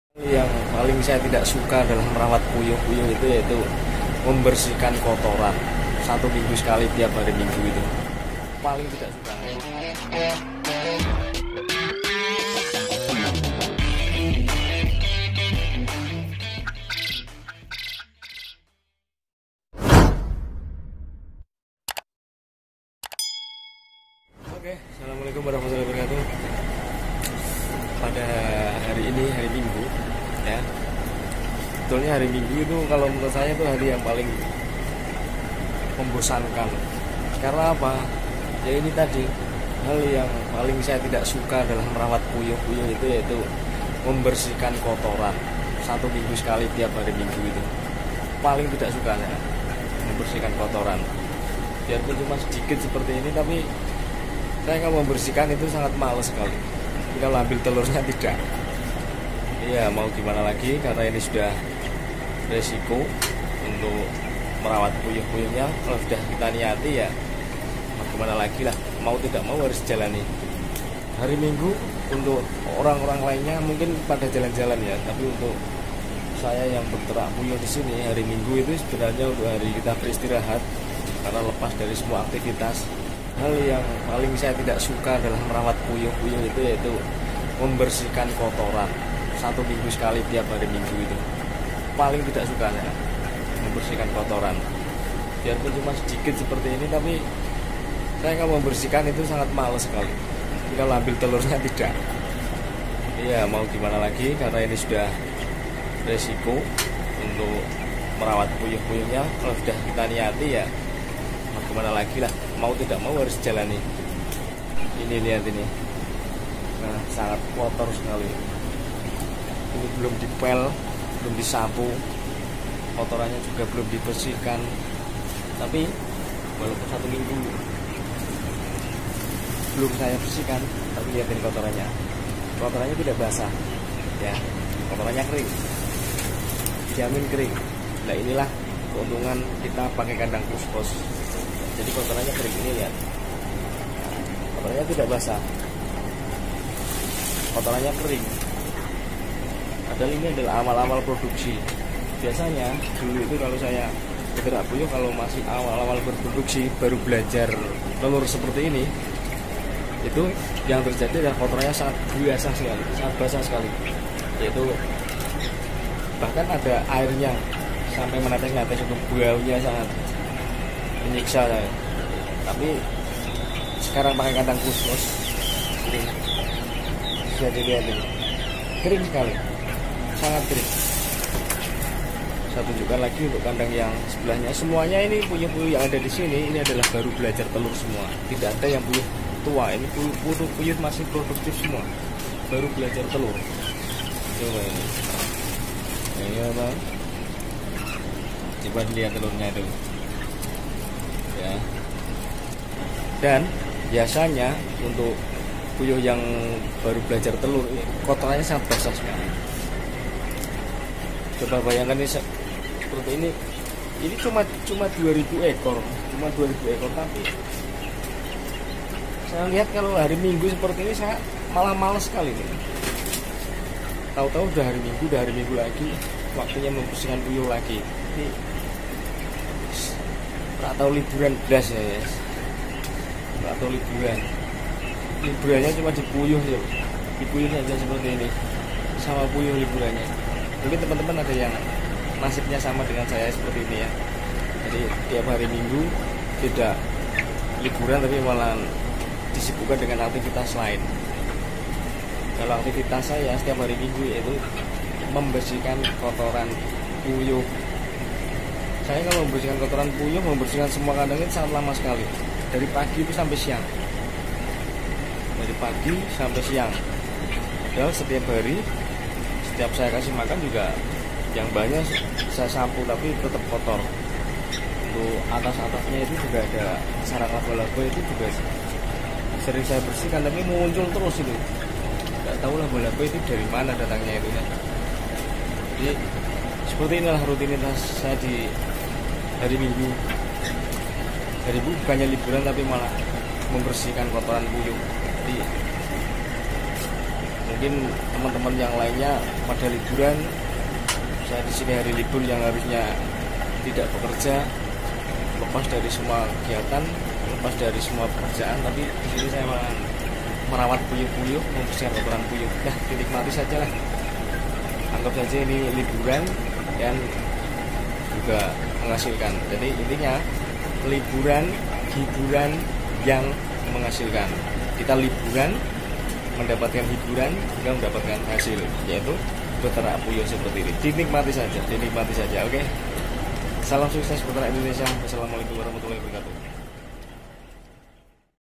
JANGAN COBA TERNAK PUYUH KALAU TIDAK MAU SEPERTI INI
Rutinitas Minggu yaitu membersihkan kotoran puyuh, hiburan yang menghasilkan..dinikmati saja dengan ikhlas biar hasil juga barokah..aminn